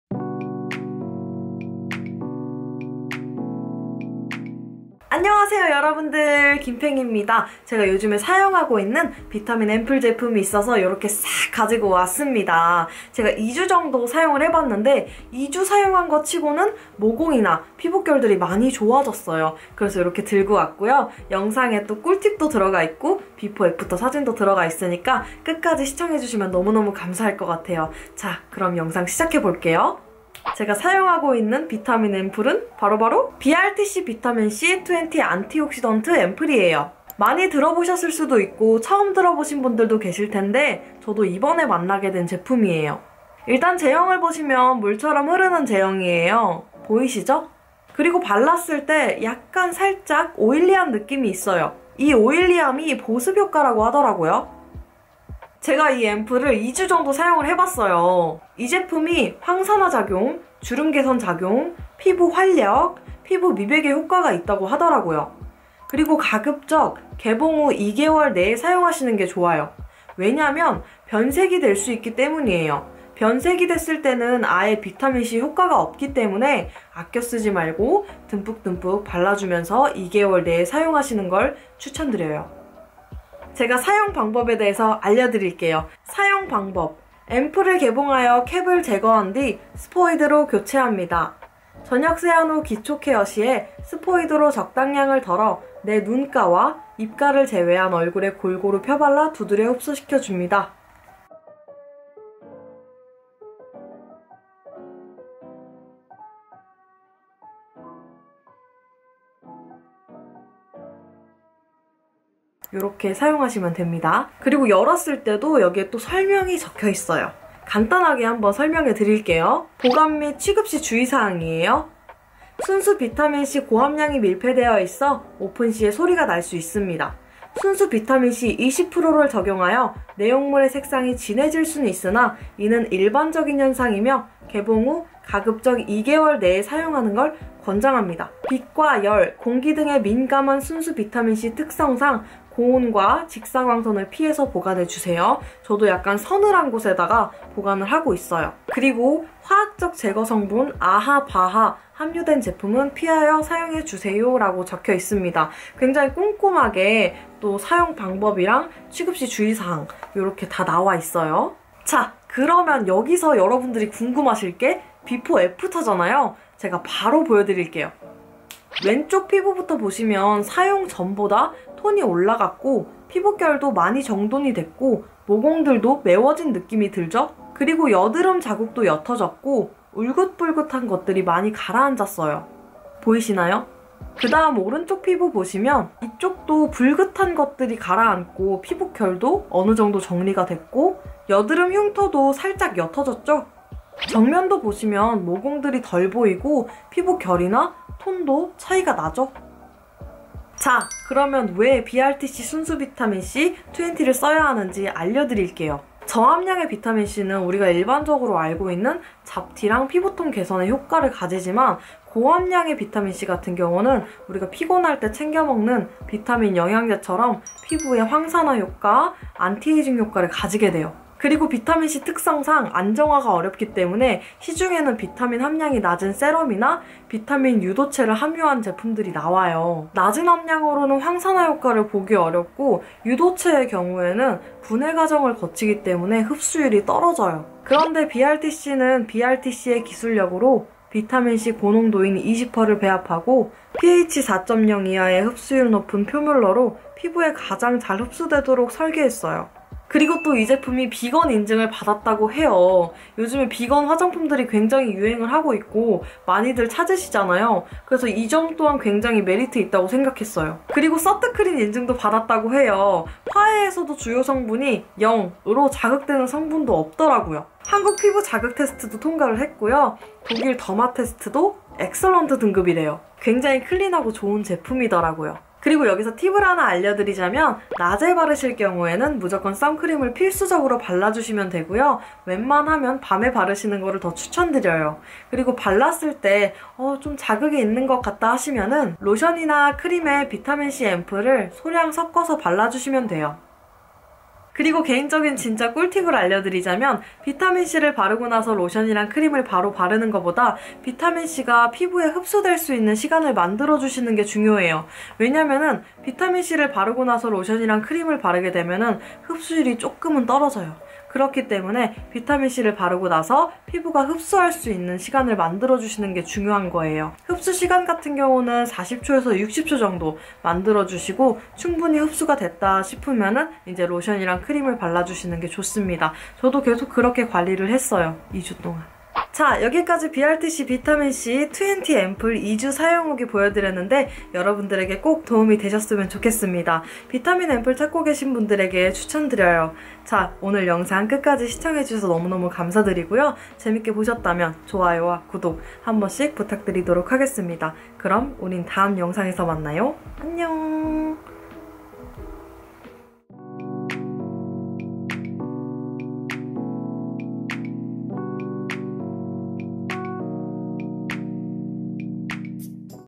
여드름 흔적,기미,잡티,피부톤업! 비타민C 세럼 2주 사용 후기! Before & After까지! 구독자 이벤트도 있어요!- [귄펭]
안녕하세요 펭귄들~ 귄펭이에요:)🐧
오늘은 비타민 앰플을 소개 드리려고 해요!
저도 이번에 만나게 되었고 2주 정도 직접 사용해 보았습니다!
자세한 내용은 영상에 담겨져 있습니다 ㅎㅎ
2주 사용 Before & After도 담겨져 있고 꿀팁과 구독자 이벤트까지!
들고 왔습니다!

[📢구독자 이벤트 내용]
비타민 앰플이 필요한 이유와 피부 고민과 제품에 대한 기대감들을
댓글로 달아 주시면 5명 추첨해 비타민 앰플을 보내드립니다!

5명이 모이면 당첨되신 분들은 '유튜브 커뮤니티' 를 통해 공지해 드릴 예정입니다!

당첨자분들은 유튜브 커뮤니티에 올려드릴 메일 주소로 공지해 드리는 내용을 보내 주시면 됩니다!

많이많이 참여해 주세요!♥

[제품 정보]

BBTC 비타민 C-20 안티옥시던트 앰플 20ml

감사합니다♥_♥